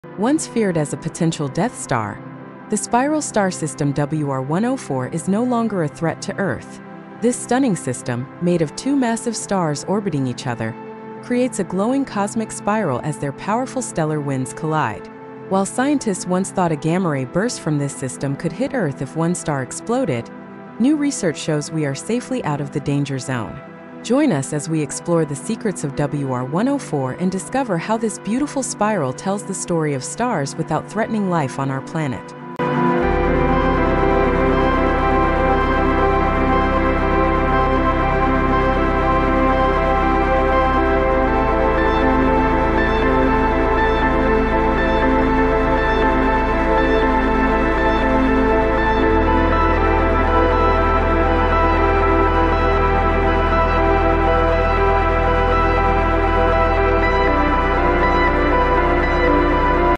WR 104’s Spiral Secrets
WR 104, known for its beautiful cosmic spiral, was once thought to be a potential “death star” for Earth, possibly sending a deadly gamma-ray burst our way. But new research confirms that this stunning binary star system is not aimed at us after all. At its heart are two massive stars whose powerful stellar winds collide, creating a glowing spiral of dust seen in infrared light. Discover how WR 104’s spiral forms, why it captured the attention of astronomers, and why we can now breathe easier knowing this cosmic giant is not a threat to life on Earth.